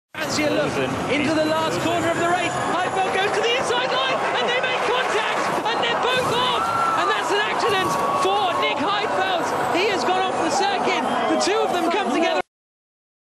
Piña espectacular en la última vuelta de la Fórmula E en Beijing.
Nicolas Prost ganaba la primera carrera de la historia de la Fórmula E. En la última vuelta, Nick Heifeld se jugó por la victoria y los dos terminaron perdiendo.